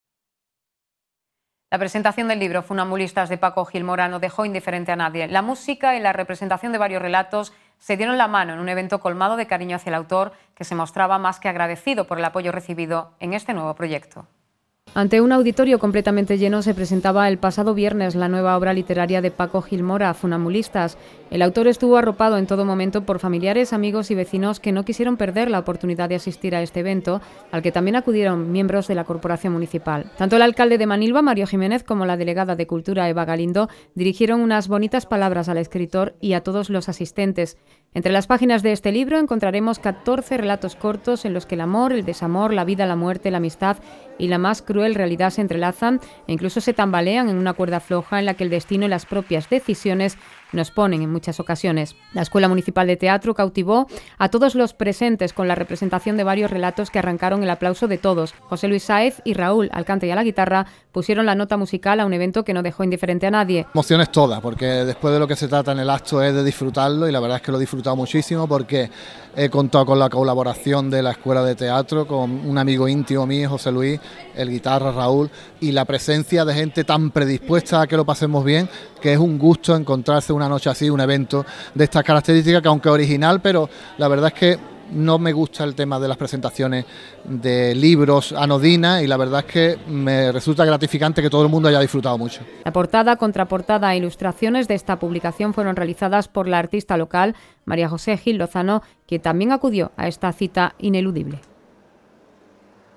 PRESENTACIÓN DEL LIBRO “FUNAMBULISTAS” DE PACO GIL MORA
Ante un auditorio completamente lleno se presentaba, el pasado viernes, la nueva obra literaria de Paco Gil Mora ,“Funambulistas”.

El autor estuvo arropado en todo momento por familiares, amigos y vecinos que no quisieron perder la oportunidad de asistir a este evento, al que también acudieron miembros de la corporación municipal.

Tanto el alcalde de Manilva, Mario Jiménez, como la delegada de Cultura, Eva Galindo, dirigieron unas bonitas palabras al escritor y a todos los asistentes.

Entre las páginas de este libro, encontramos catorce relatos cortos en los que el amor, el desamor, la vida, la muerte, la amistad y  la más cruel realidad se entrelazan e incluso se tambalean en una cuerda floja en la que el destino y las propias decisiones nos ponen en muchas ocasiones.

La Escuela Municipal de Teatro cautivó a todos los presentes con la representación de  varios relatos que arrancaron el aplauso de todos.

José Luis Sáez y Raúl, al cante y la guitarra, pusieron la nota musical a un evento que no dejó indiferente a nadie.

La portada, contraportada e ilustraciones de esta publicación fueron realizadas por la artista local, María José Gil Lozano, quien también acudió a esta cita ineludible.